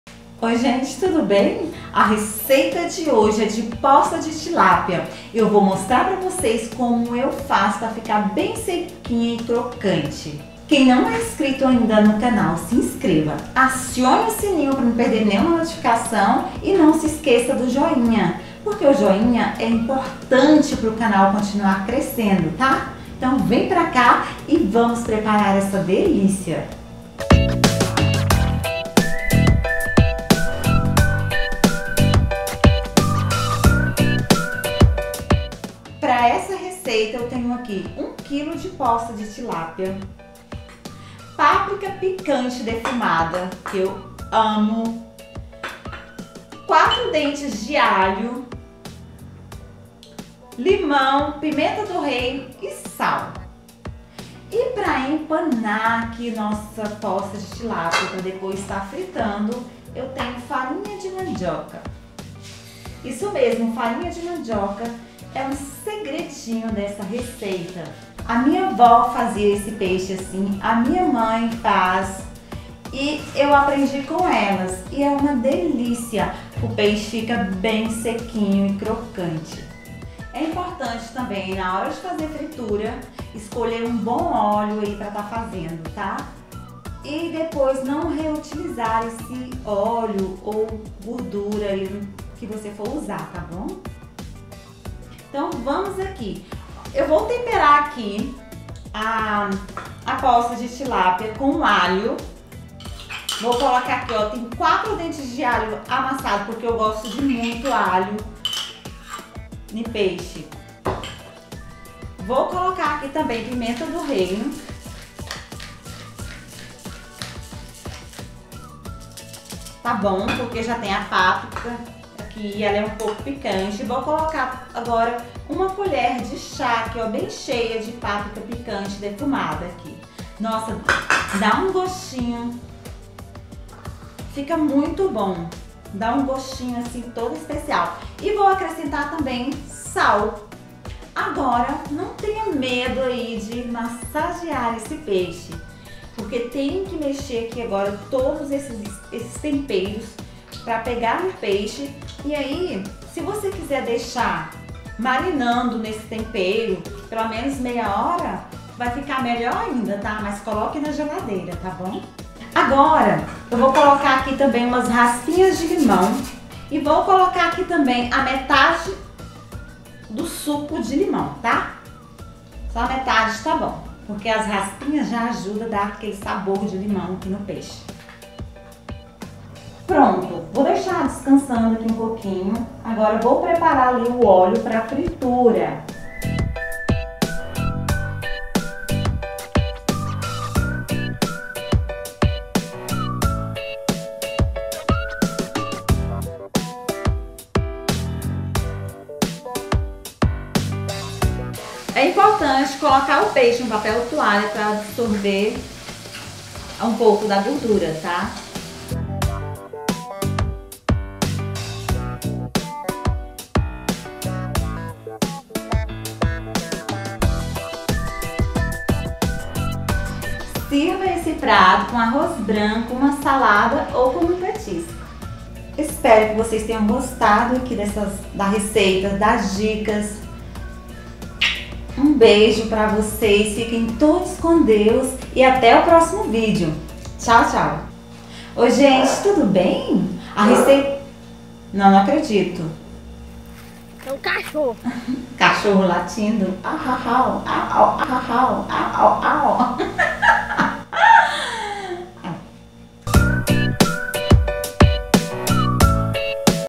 Posta de Tilápia Crocante Empanada Com Farinha De Mandioca, Fácil De Fazer
Ingredientes;
1 kg de posta de Tilápia
4 Dentes de alho
sal a gosto
Páprica  picante defumada uma colher de chá
Pimenta do reino  a gosto
Meio limão
Farinha de mandioca